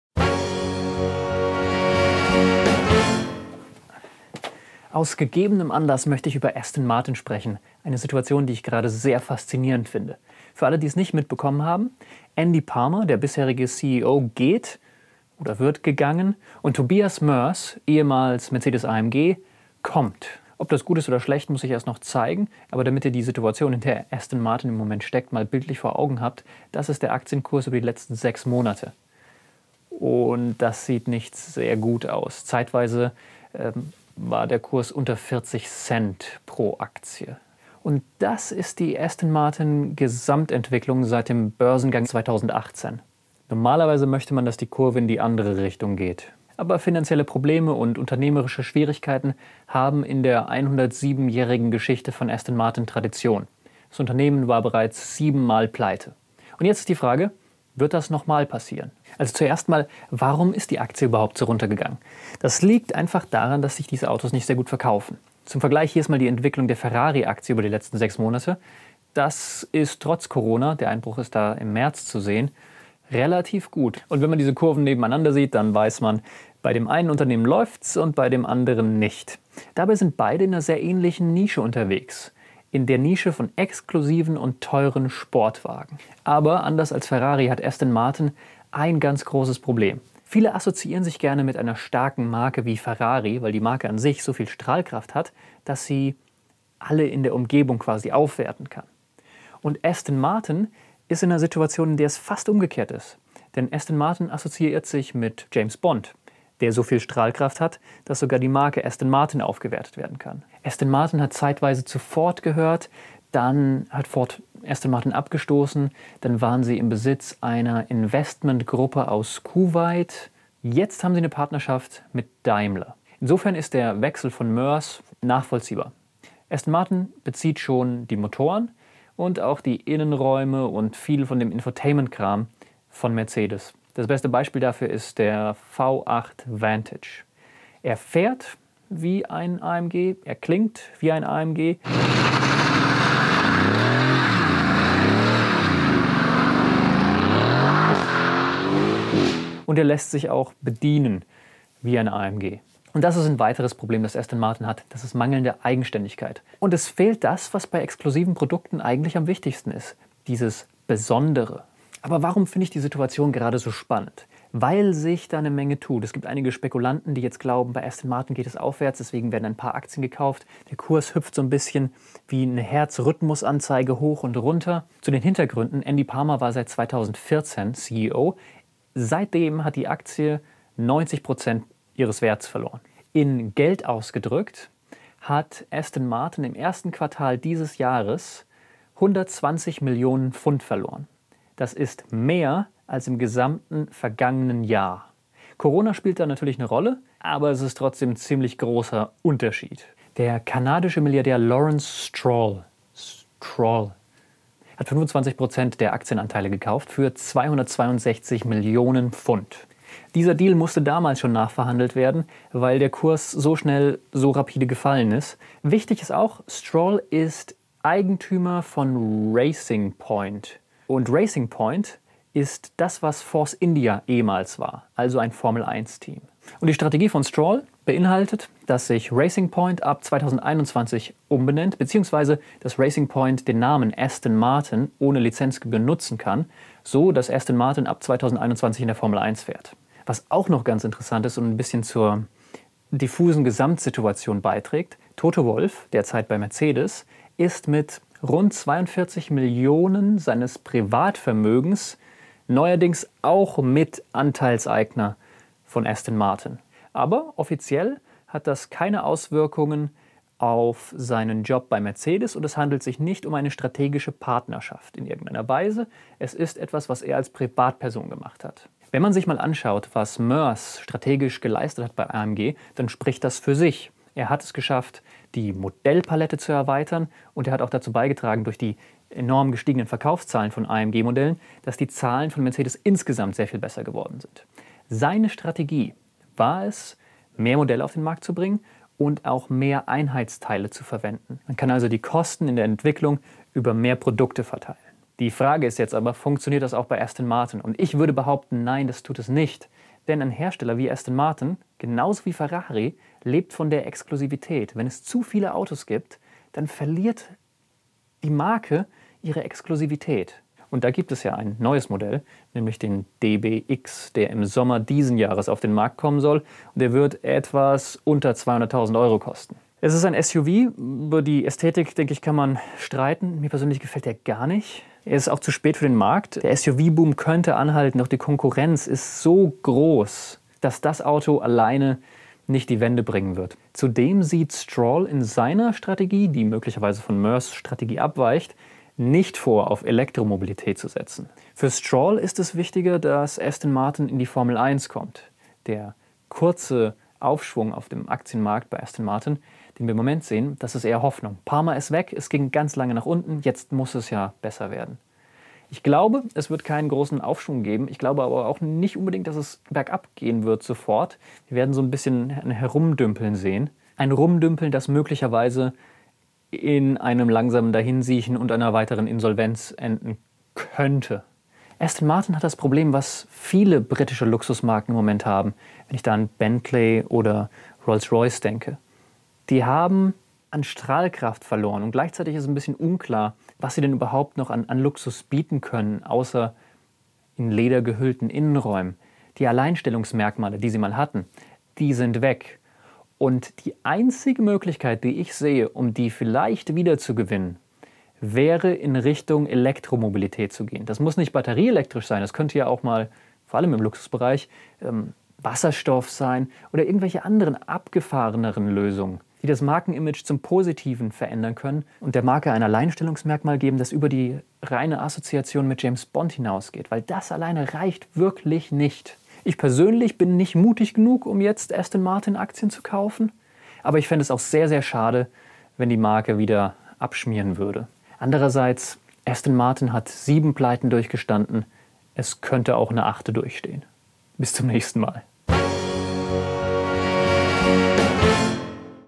Pleite? Geht Aston Martin bald unter? | BLECH REDEN
An der Spitze von Aston Martin wird Andy Palmer von Tobias Moers ersetzt. Das sorgt für reichlich Tumult beim Aktienkurs des Unternehmens. Zur Zeit im Positiven. Doch das liegt wohl eher an der Hoffnung der Anleger und weniger an echter Verbesserung der Lage. Dafür ist es ohnehin noch zu früh. Aston Martin steckt mal wieder in der Krise und noch ist fraglich, ob die Luxusmarke da so schnell wieder rauskommt, oder ob es in der Pleite endet. Es wäre die achte in der 107-Jährigen Unternehmensgeschichte. Es gibt viele Fragen! Einige Antworten bekommt ihr im Video.

Mehr über alles zum Thema Auto, erfahrt Ihr auf unserer

Unser Video-Equipment:

*Objektiv 1
*Objektiv 2
*ND Filter
*Pol Filter